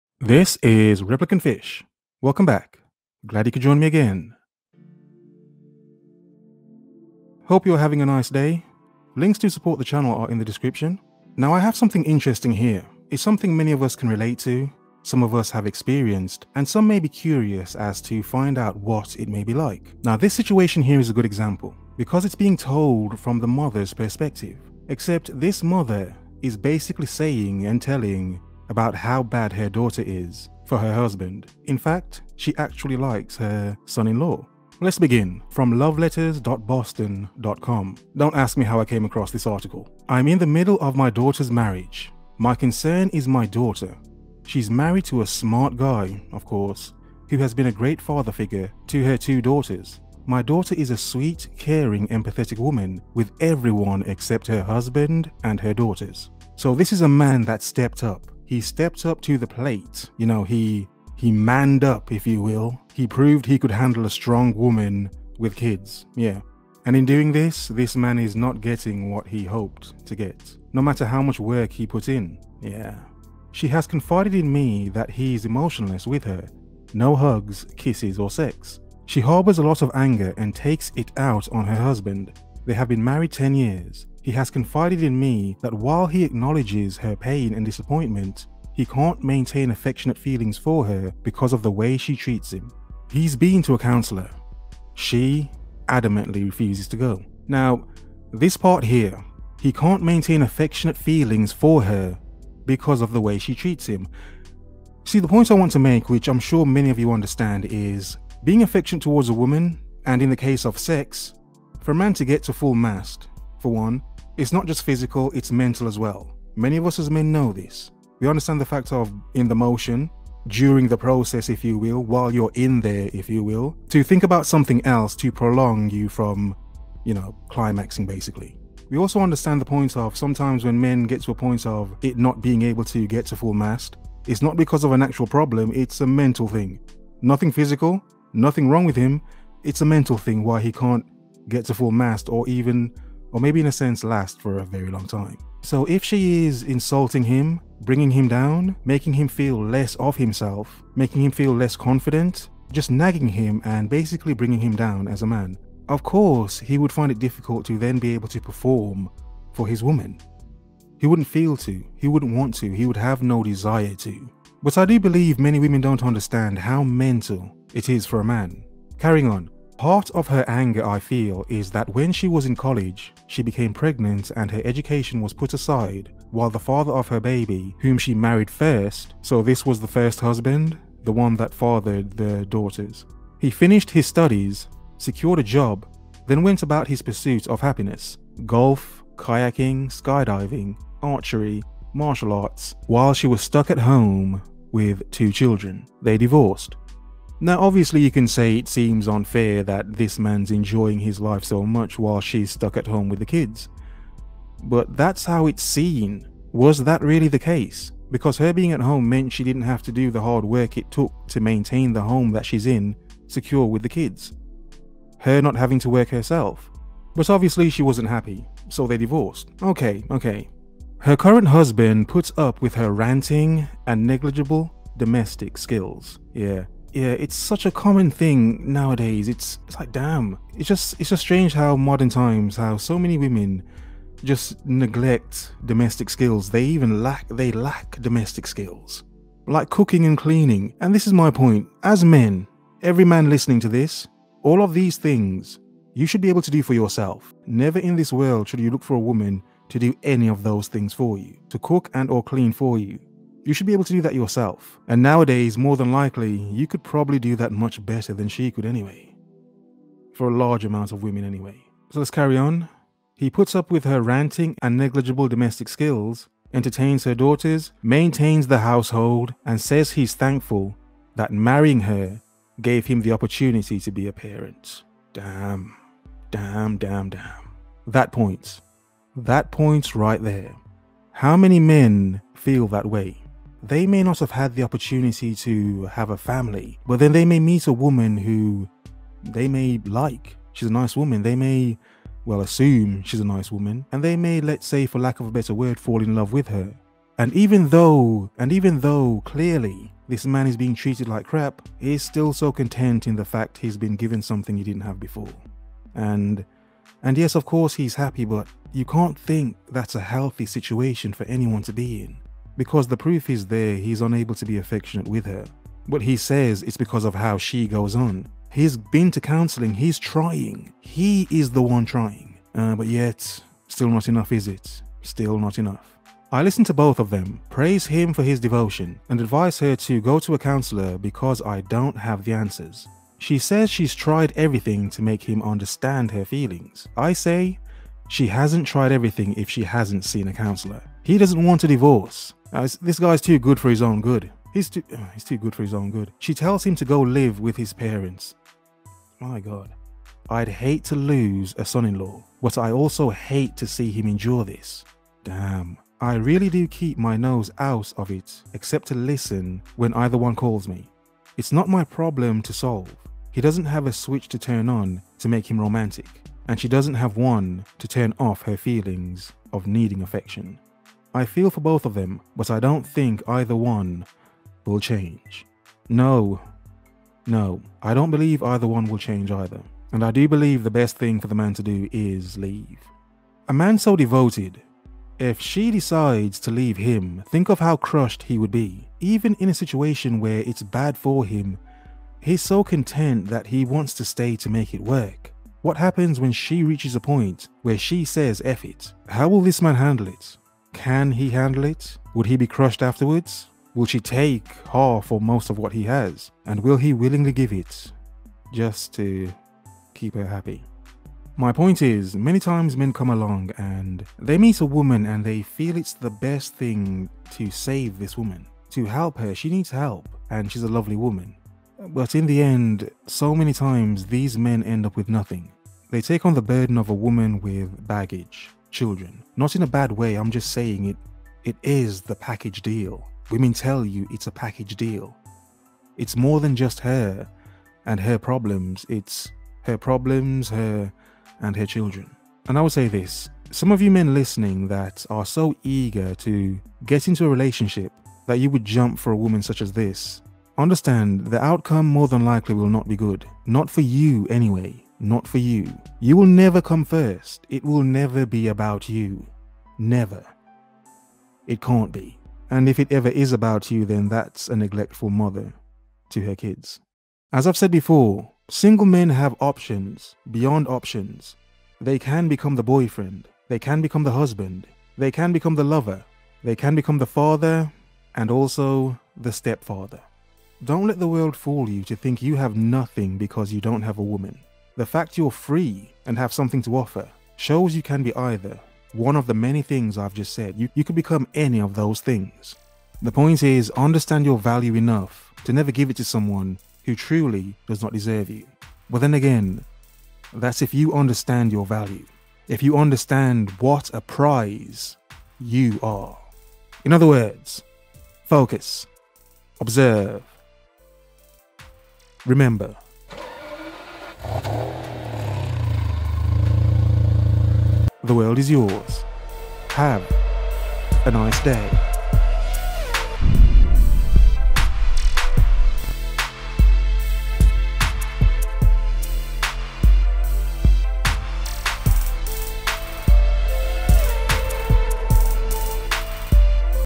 An Example Of The Good Man That Stepped UP and Got Stepped ON
Here we have a story about a woman who thinks her daughter is destroying her son in law, but he refuses to divorce her. Even though he is a good stepfather to her kids and cleans and maintains the home, she still treats him like crap. A single mother also. An Example Of The Good Man That Stepped UP and Got Stepped ON

Advice about life, dating and women.